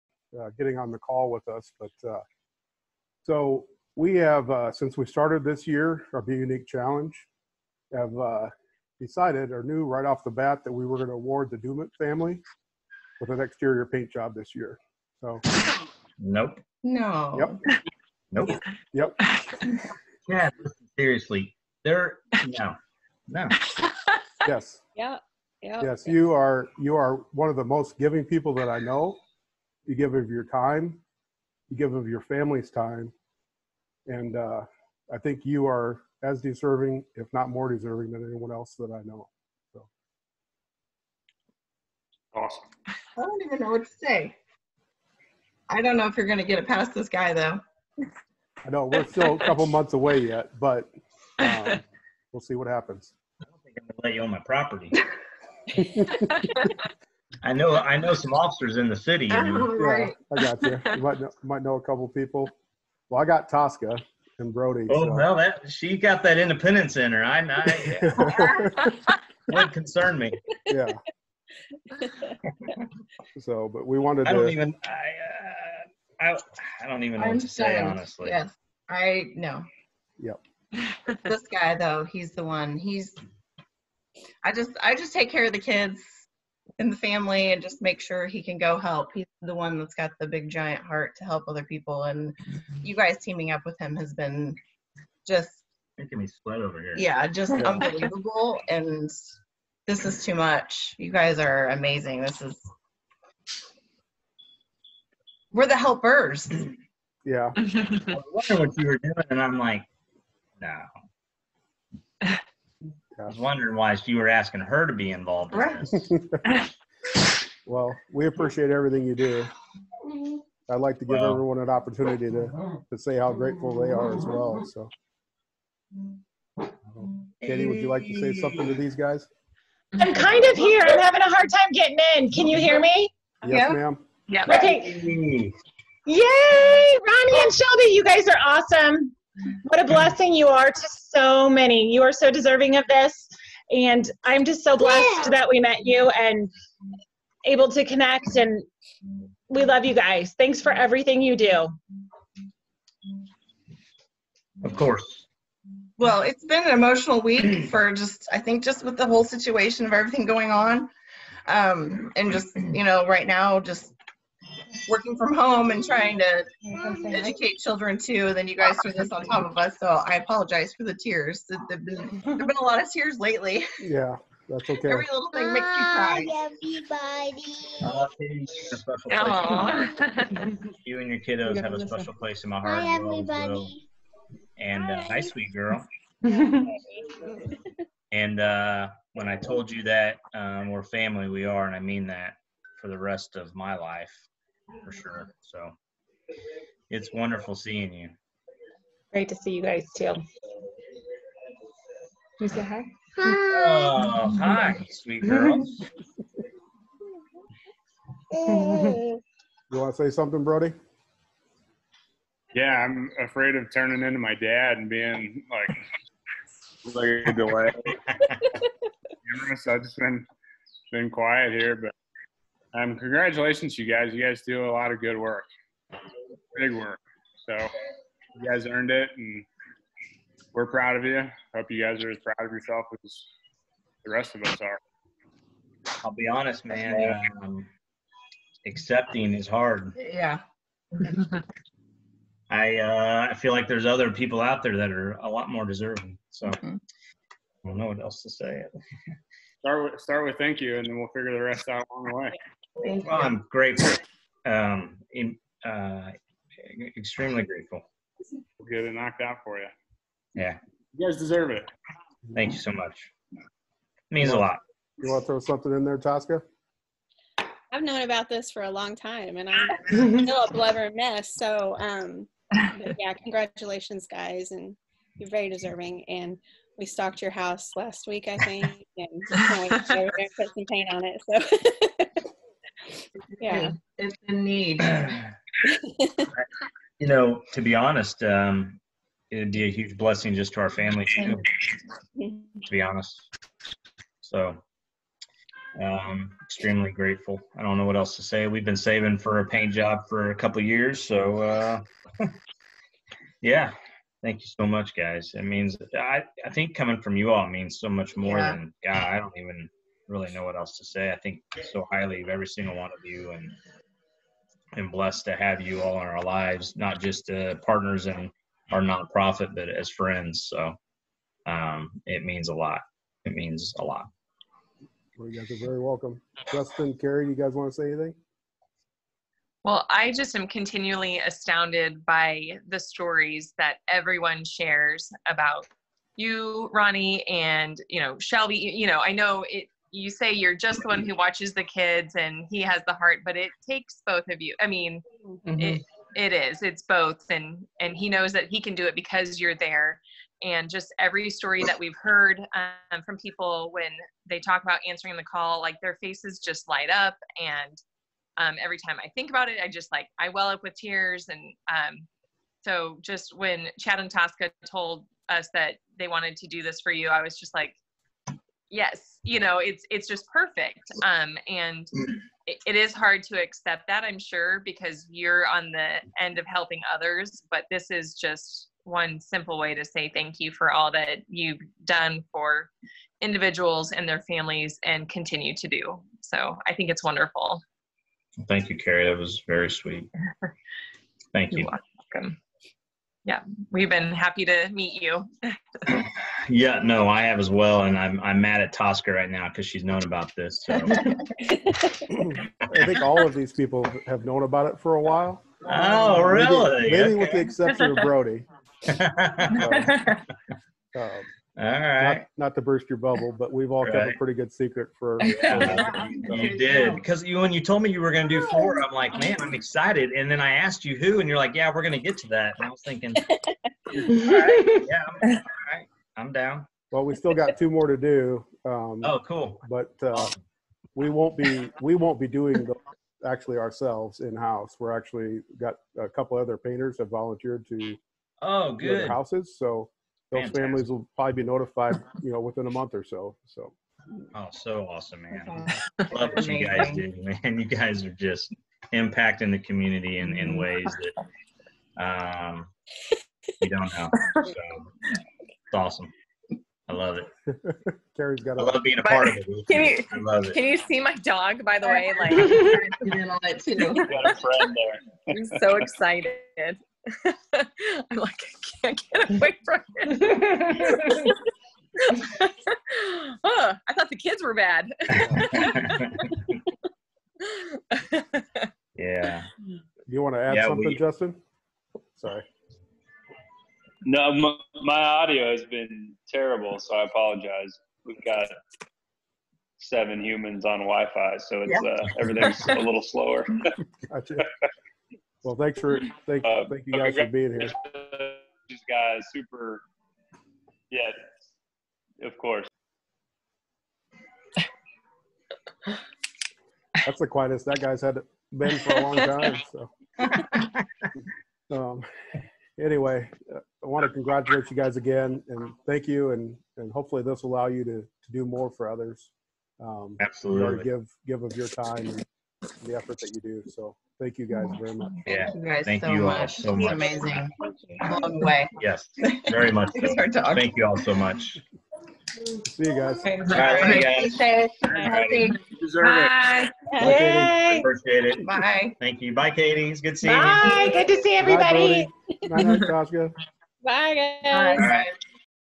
Be Unique Challenge 2nd Giveaway Reveal
Ronnie Doumitt started the foundation, Answering the Call, and we have been so fortunate to be linked up with him through our Be Unique Challenge. He gives so much of his time and his heart to helping first responders in their time of need, and we knew he was deserving of a free exterior paint job as well.

Last week, we were able to surprise Ronnie and his wife Shelby, and we'd be lying if we said we all didn't shed a tear.